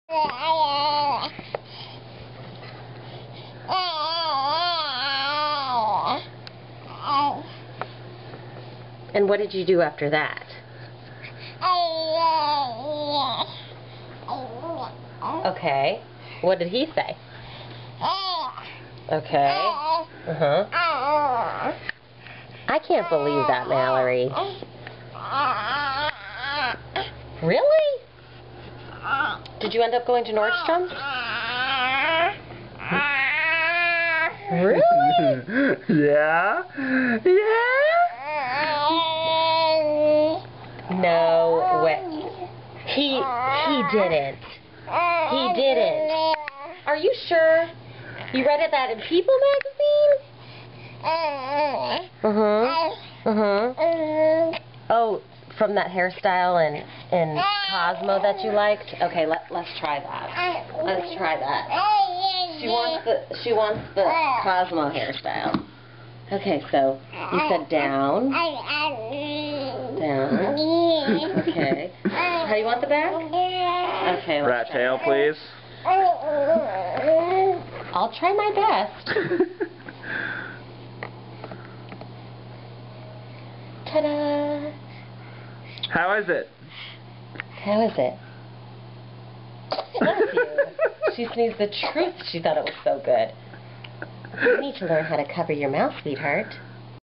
Talking
Mallory having a great conversation with her Mama